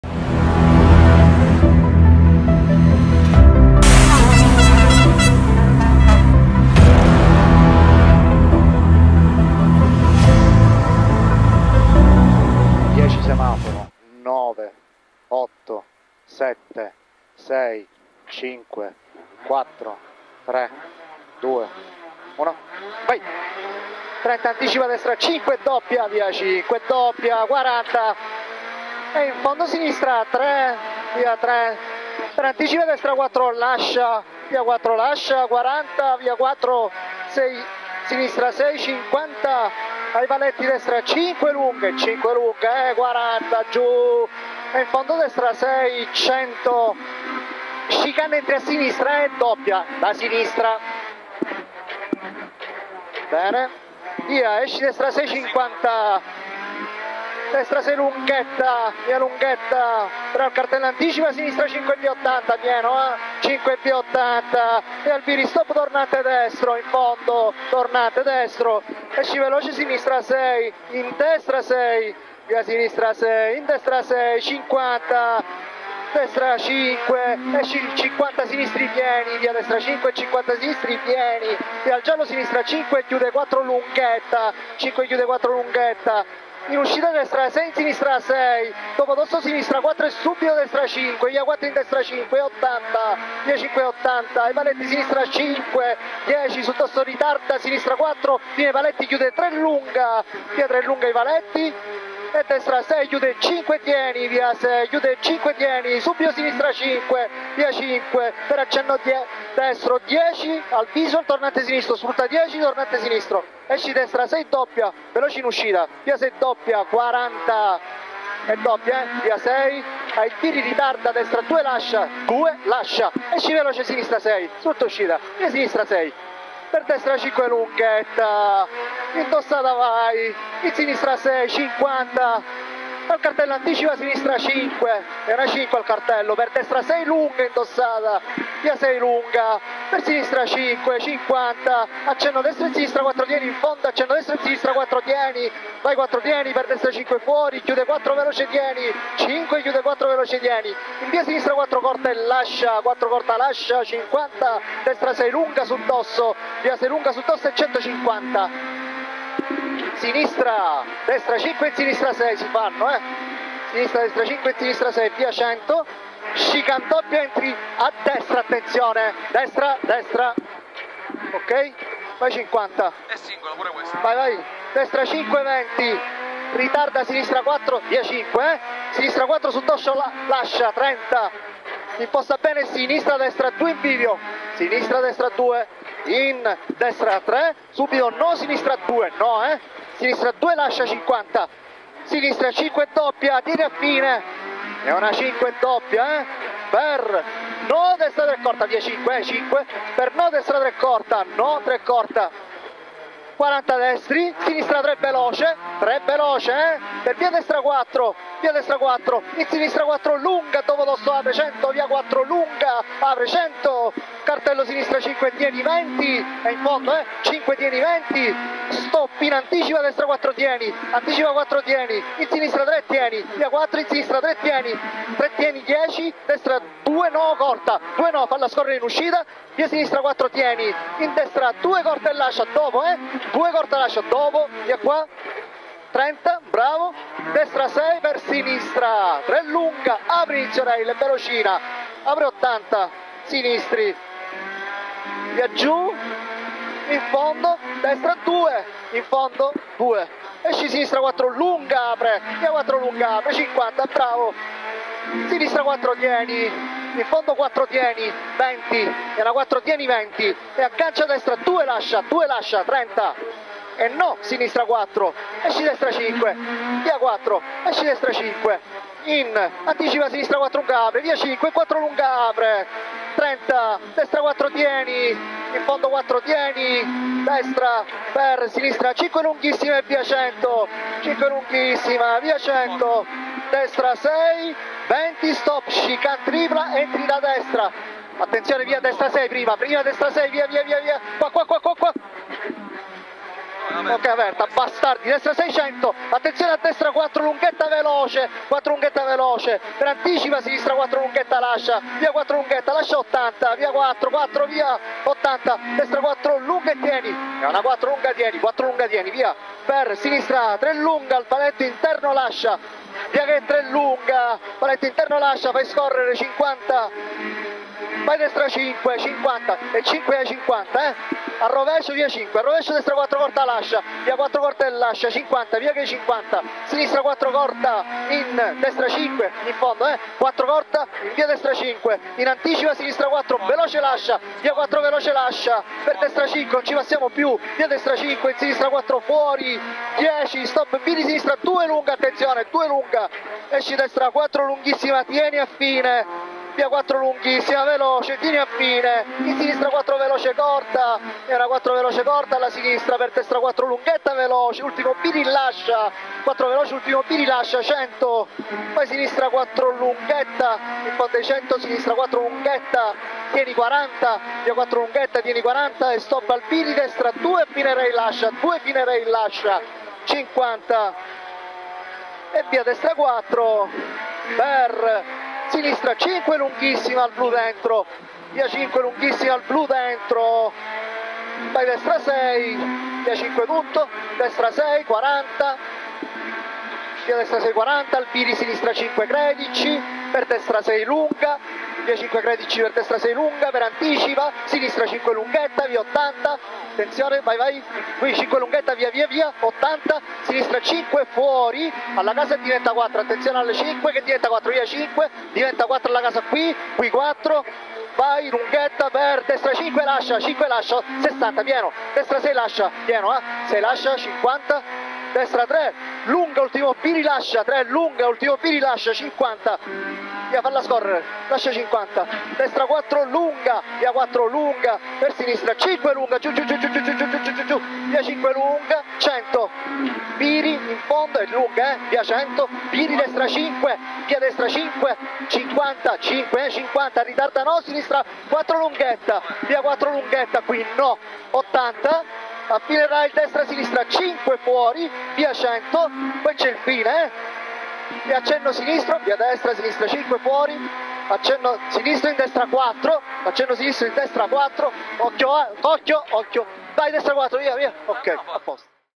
Cameracar RIZZO PITTELLA 28' Rally del Rubinetto 2021
Ps 4 - Mottarone su Peugeot 208 R2B